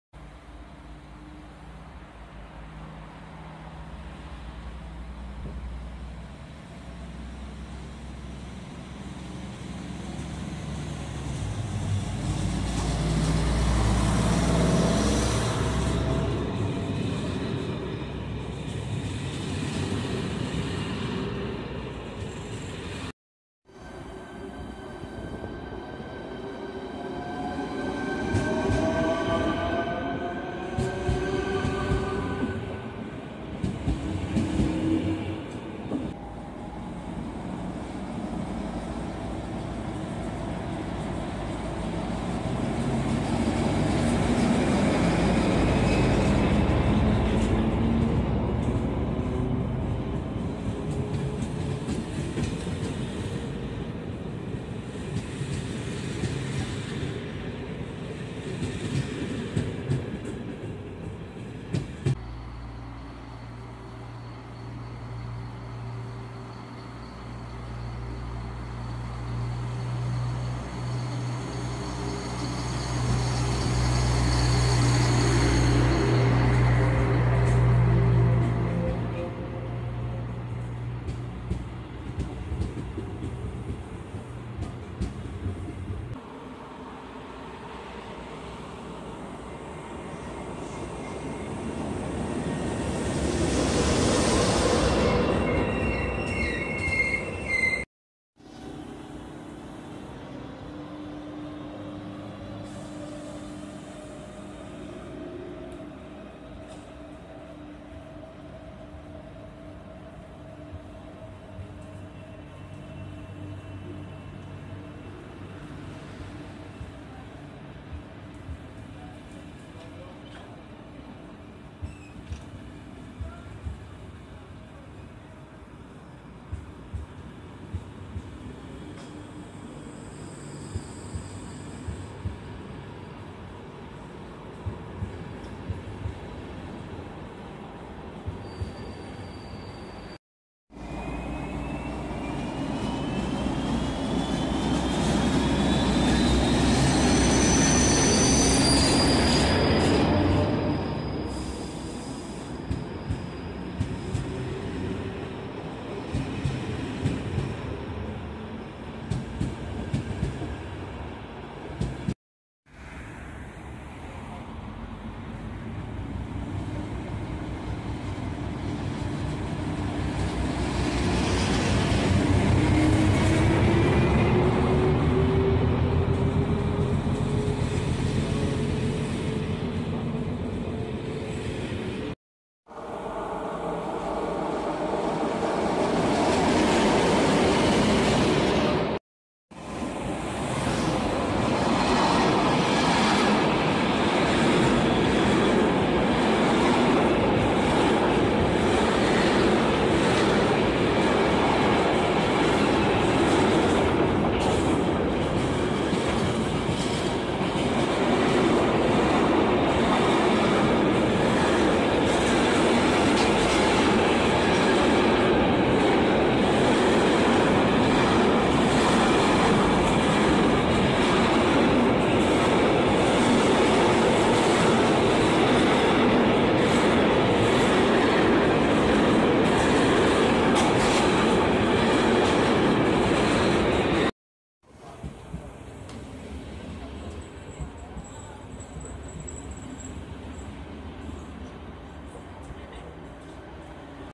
Vlaky Ostrava-Stodolní • 10.10.2019
Sestřih videi ze zastávky Ostrava-Stodolní pocházející z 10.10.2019.
0:00 - Os 3136 (Ostrava hl.n. - Frenštát pod Radhoštěm)
0:23 - Sp 1619 (Ostrava-Stodolní - Český Těšín)
0:36 - Os 3135 (Frenštát pod Radhoštěm - Ostrava hl.n.)
1:03 - R 1127 "Praděd" (Olomouc hl.n. - Ostrava střed)
1:26 - RJ 1005 (Praha hl.n. - Havířov)
2:20 - Os 3158 (Ostrava hl.n. - Frýdlant nad Ostravicí)
2:42 - Os 3157 (Frýdlant nad Ostravicí - Ostrava hl.n.)
3:03 - Lokomotivní vlak
3:09 - Nákladní vlak
3:53 - Sp 1622 (Český Těšín - Opava východ)
Tratě na zastávce: 321, 323
Informační systém: INISS

• VYBAVENÍ
Mobil – Asus Zenfone Go